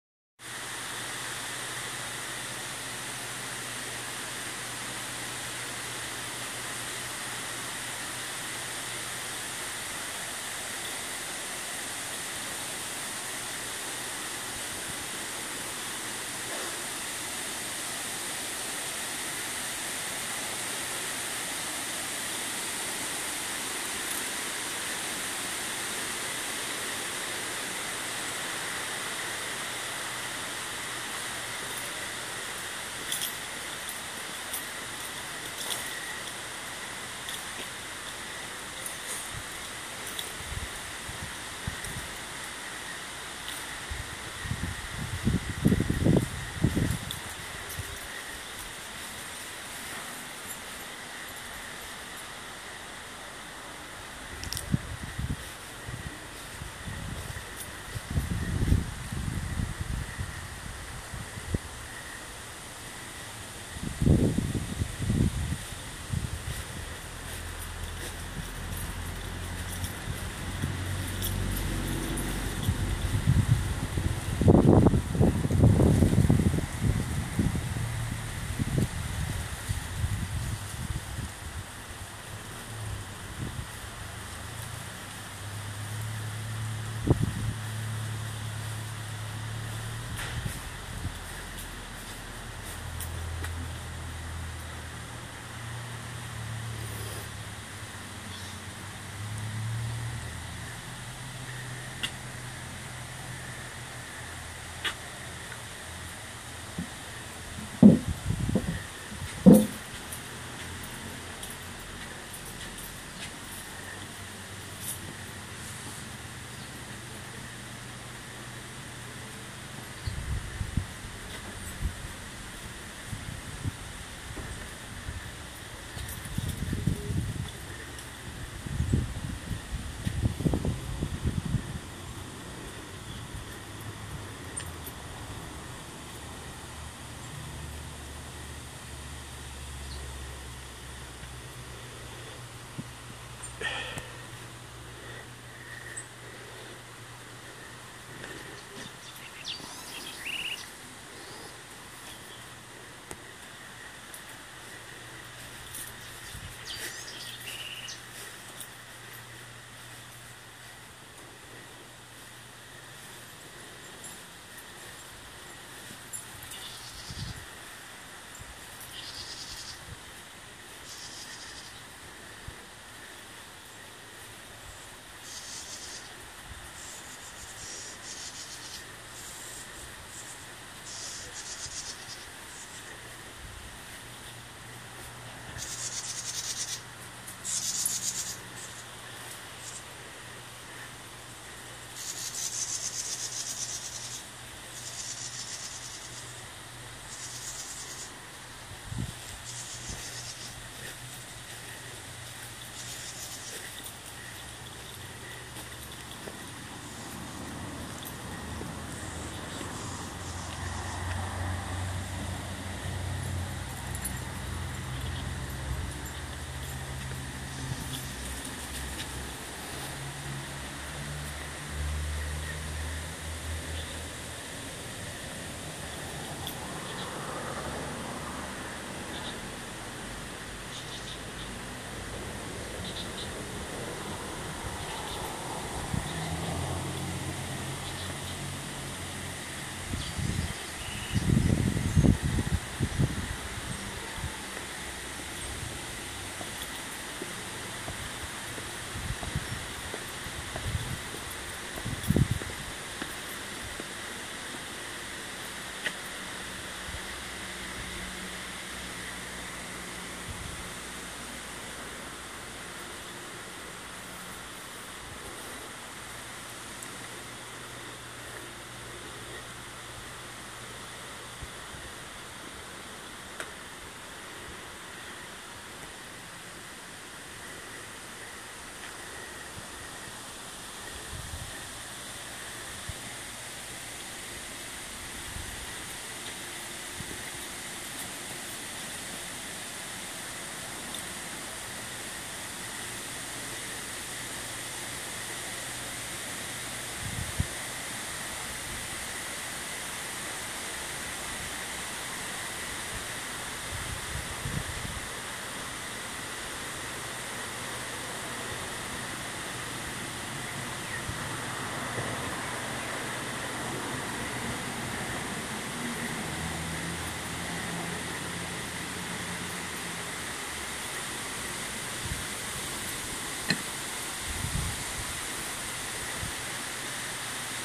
Beginning of the green season.
This is our yard in Costa Rica. It is the beginning of the rains and the plants are coming alive. In a couple of more weeks, our garden areas will be a sea of living colors. I will try to repost.